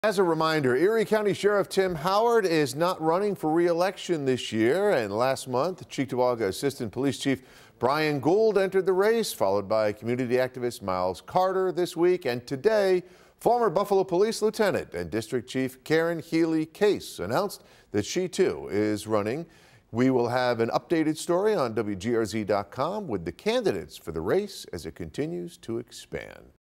3 WNYers launch campaigns to run for Erie County Sheriff
Karen Healy-Case on Wednesday joined Myles Carter and Brian Gould in announcing her candidacy for the post.